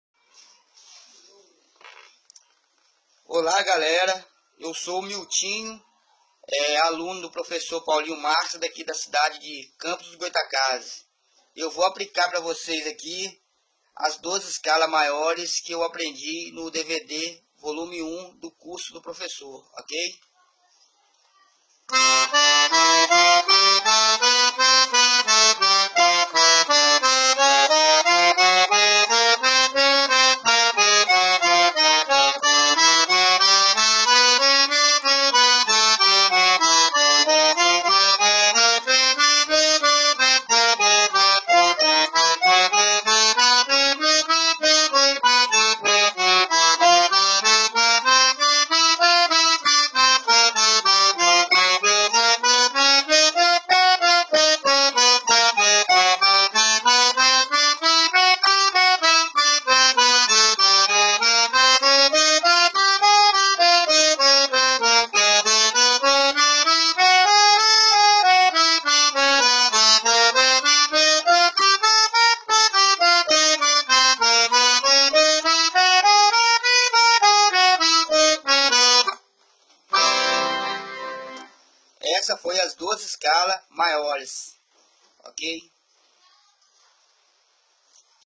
Acordeon - Depoimento de aluno - campos dos goytacazes - RJ - Prof: Paulinho Marques
Acordeonista e aluno: Miltinho - campos dos goytacazes - RJ

APRENDA AGORA!!! - Acordeon - sanfona - gaita piano.
Paulinho Marques – Educação Musical de Qualidade.

Conheça meus cursos para: ACORDEON – SANFONA – GAITA PIANO.